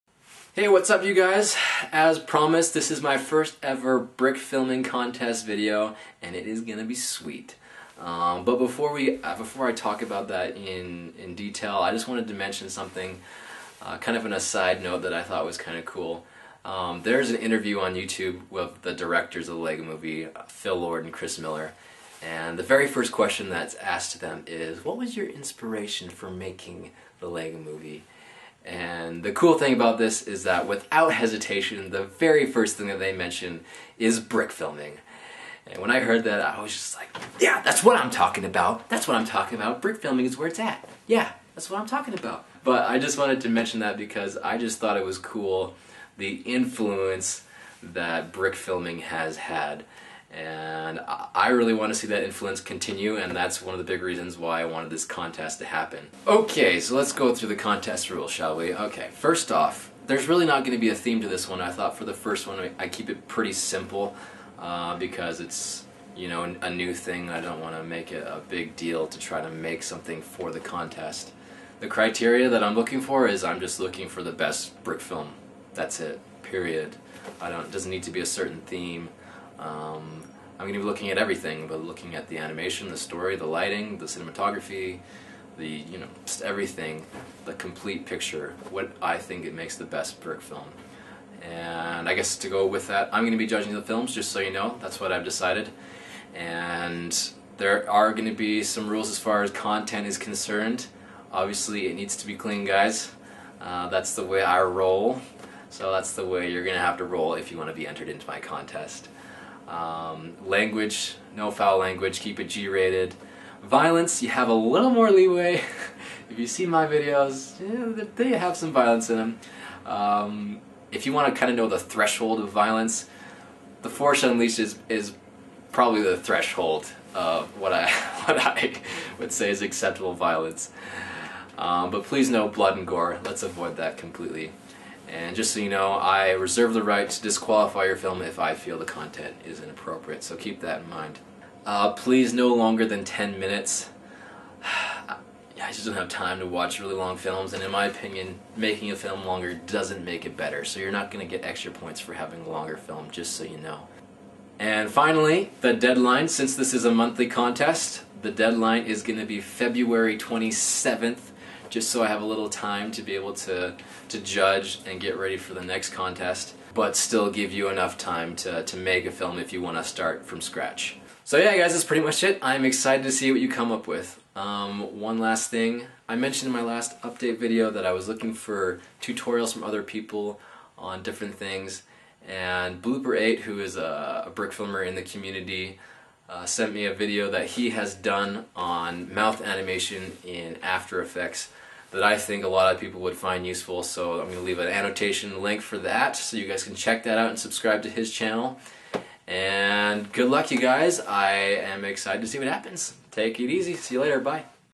February Brickfilming Contest!
CONTEST CLOSED!  Thanks to everyone who entered, and for those who didn't get a chance, look out for the next contest coming soon!

Here are the contest guidelines:

- Your film does NOT have to be made in February.  It can be older.
- Maximum time limit is 10 minutes
- Language must be G - rated
- Violence is allowed but no blood and gore
- Submit your video (by sending me a link via pm) by FEBRUARY 27, 2014

Good luck and I can't wait to see what you guys come up with!

I learned about this interview from The Set Bump website, check them out too!